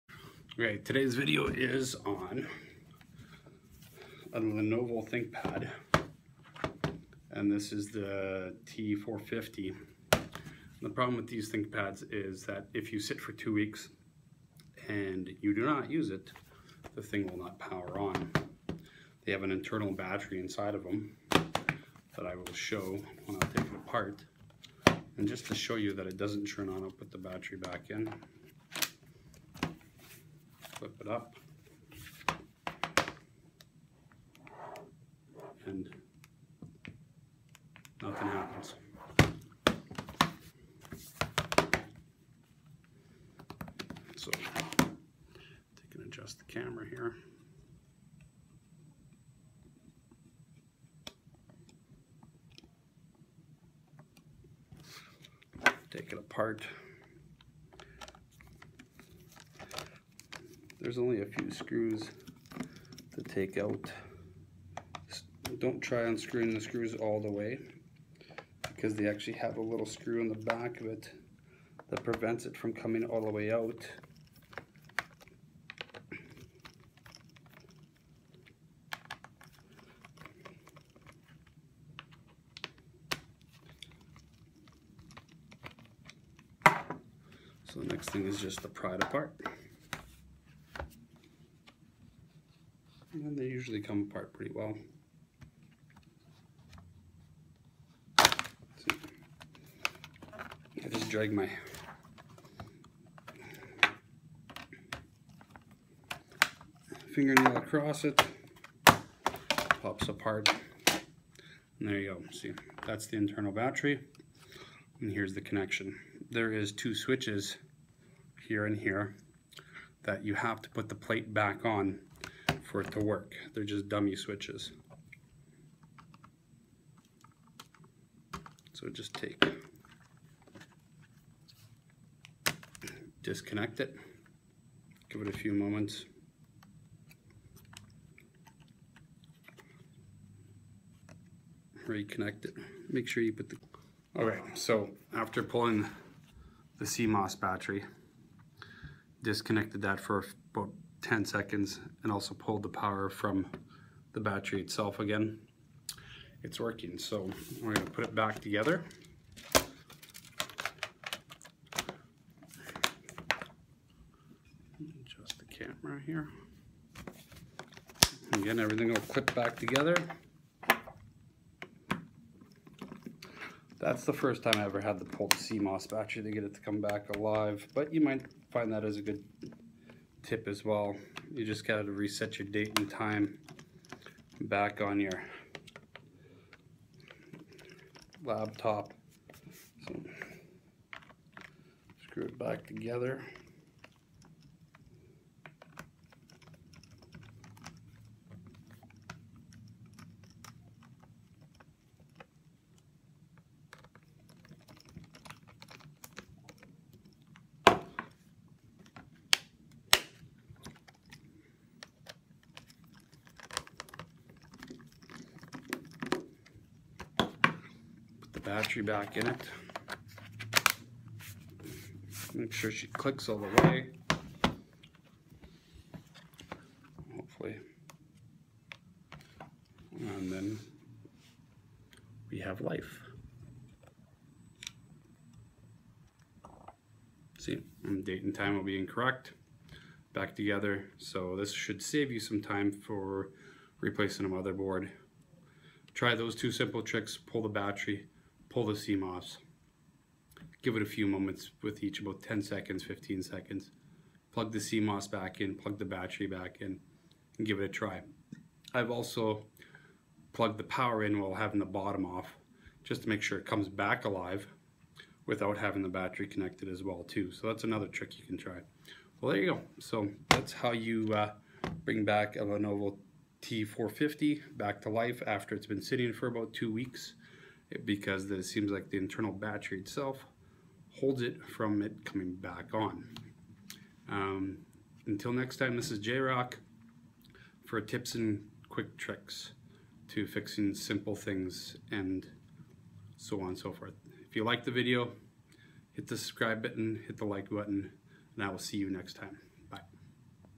Lenovo T450, T450s, X250, L420 and T440S Not Powering ON Quick Fix
I DO NOT GET PAID BY LENOVO OR ANY SUPPORT BY LENOVO AS A COMPANY. THIS IS A QUICK HELPFUL TIP. PLEASE CONSIDER DONATING. THANK YOU!

Come back from holiday and your new lenovo thinkpad doesn't power on try out this trick and it could work for you! remember these are just quick fixes that may NOT work for you but worth a try.